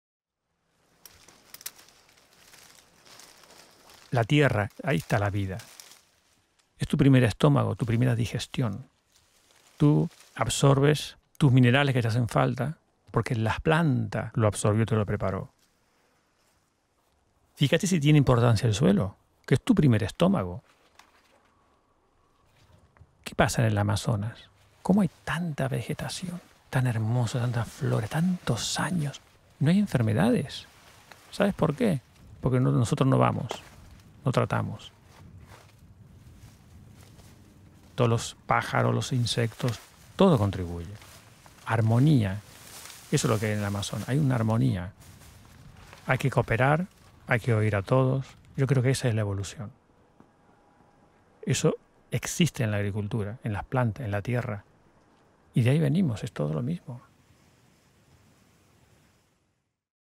"Why there is no illness in the Amazon" | OSCAR by Human Postcards
SPAIN | Oscar grew up on a farm in Argentina. During the great depression he moved to Europe and became an IT specialist. At the age of 50 he felt the need to go back to his traditions, the earth, farming. With this sudden urge to reconnect with nature he purchased some land in Alicante and start an organic farm. He approaches farming in a very scientific yet holistic way. He studies biology and has a very deep understanding of nature's cycle and how to work with it. This rich video portrait taught us about the importance of respecting nature's rhythm.